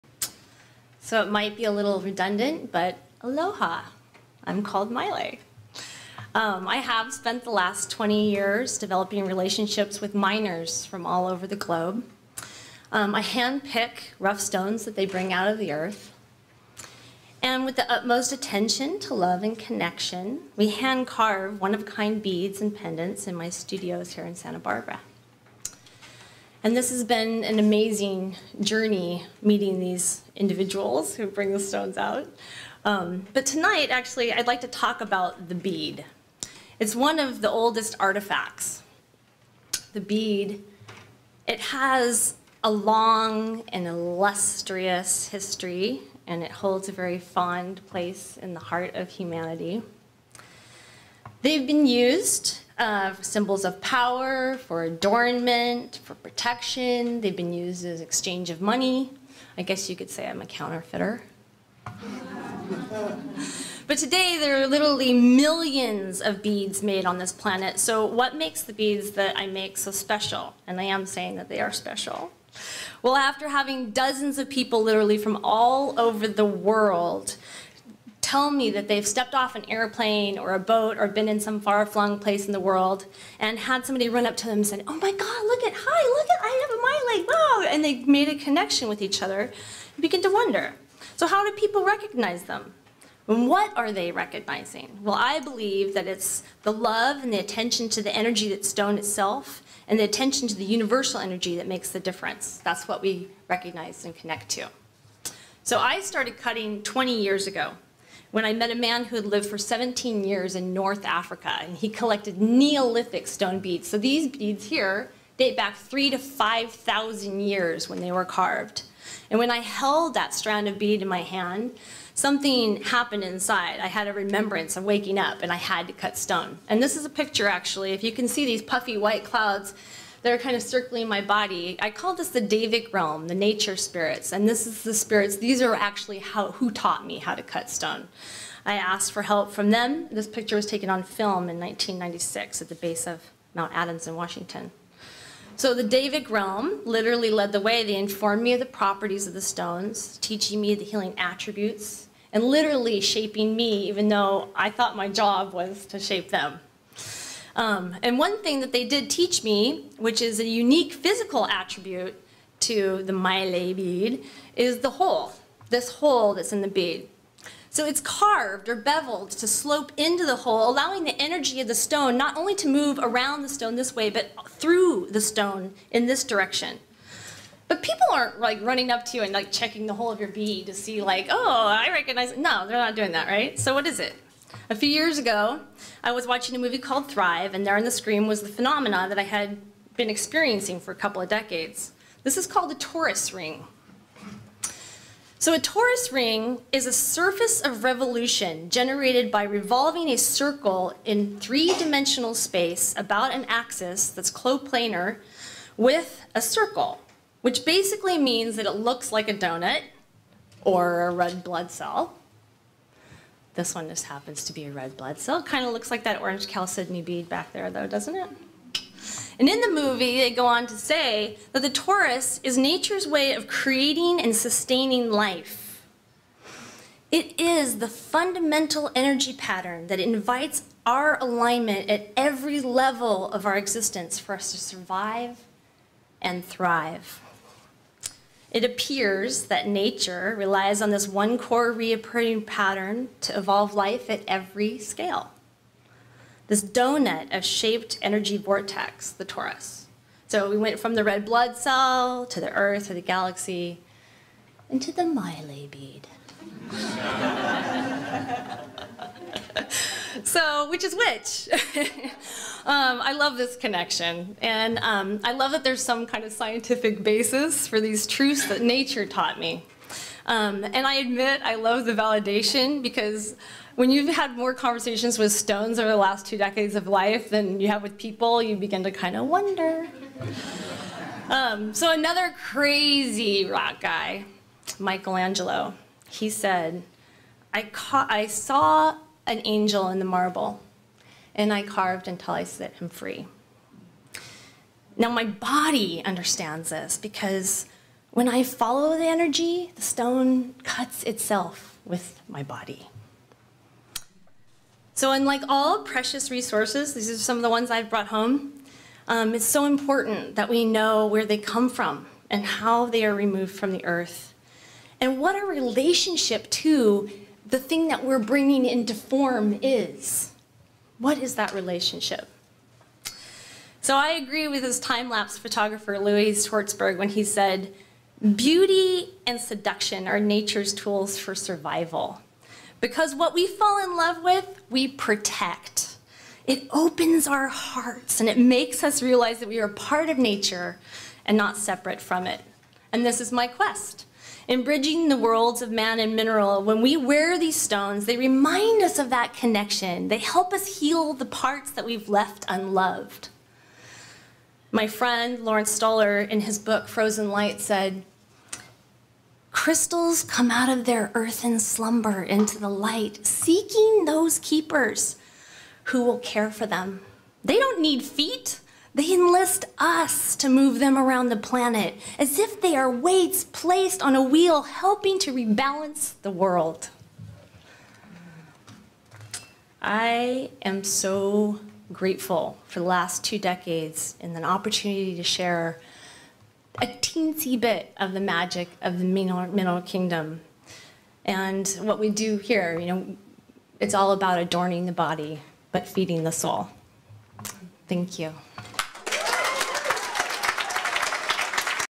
Maile Ellington: The Amazing Magic of the Mineral Kingdom
A journey into the amazing magic of the mineral Kingdom, the ancient art of hand carved stone, and the secret of man kinds fascination with the bead.

Maile was born with a deep affinity for nature. Twenty years ago this passion found a profound expression when Maile was entranced by the wonders of Mother Earth's minerals. Since that time she has worked at perfecting every aspect of creating exquisite art out of stone. She handpicks her rough materials, choosing the most rare and fine from all over the world. She is conversant with the vibrational properties and inherent character of each stone. She attends to her carving with the deepest intention of producing jewelry that adorns the body and feeds the soul. The result is outrageously beautiful hand carved stone jewelry that lovingly keeps alive the art of lapidary.